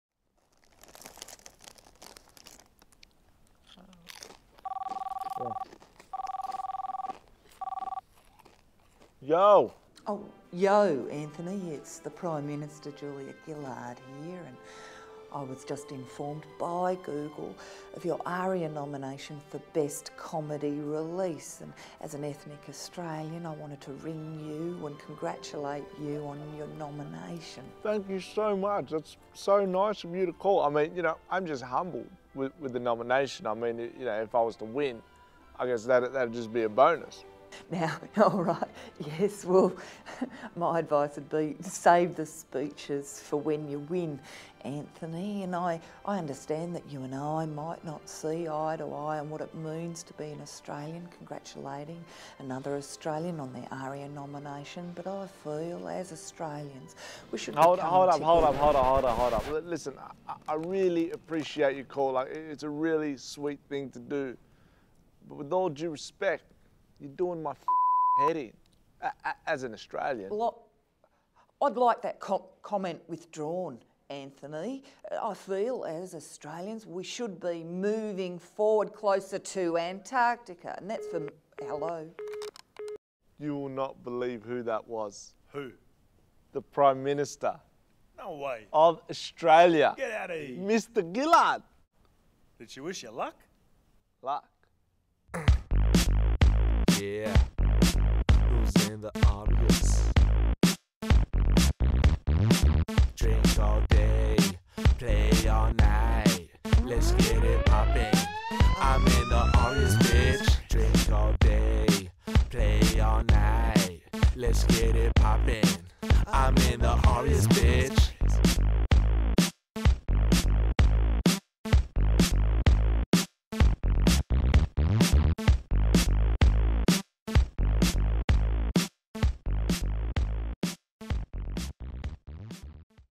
Julia Gillard congratulates Anthony Salame on Aria nomination.
Follow Anthony on Instagram, Twitter and Facebook @anthonysalame

This is what happened when Prime Minister of Australia Julia Gillard called Anthony to congratulate him on his ARIA nomination!

Music recorded and produced by SouthWest Ent.

Julia Gillard played by Gabby Millgate.